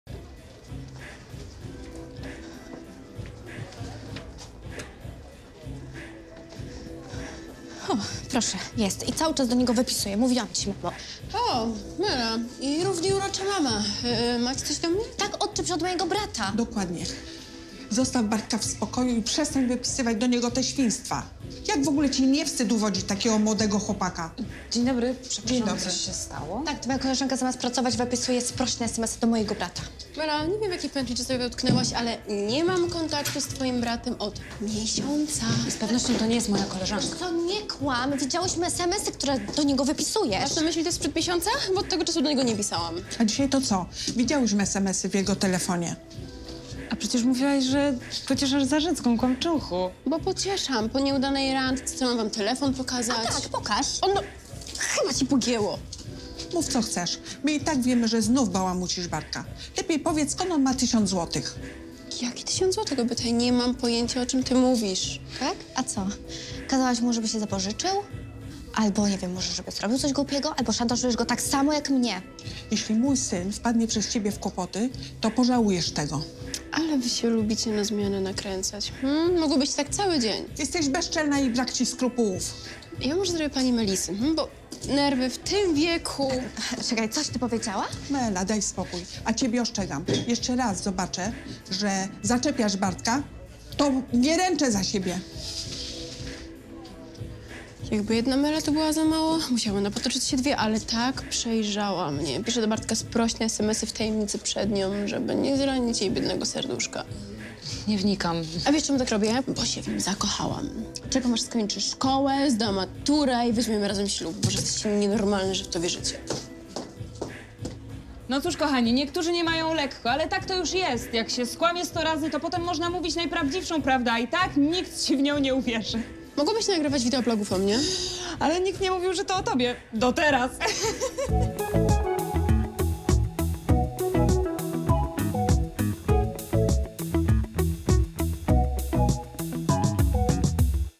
Mela zażądała, żeby Dominika natychmiast odczepiła się od jej brata!
Iwona została wolontariuszką w fundacji opiekującej się starszymi osobami. Tego dnia spotyka się z panią Zofią, której zamierza dotrzymać towarzystwa i pomóc. Razem wybierają się na zakupy, gdzie Iwona spotyka Wojtka. Niespodziewanie w sklepie pojawia się uzbrojony mężczyzna, żąda pieniędzy i sprawia wrażenie nieobliczalnego. Wojtek, Iwona oraz pani Zofia staja się zakładnikami., Wkrótce dochodzi do tragedii… Tymczasem Mela i jej mama martwią się o Bartka, u którego znalazły dużą sumę pieniędzy. Podejrzewają też, że nastolatek wciąż utrzymuje kontakty z Dominiką.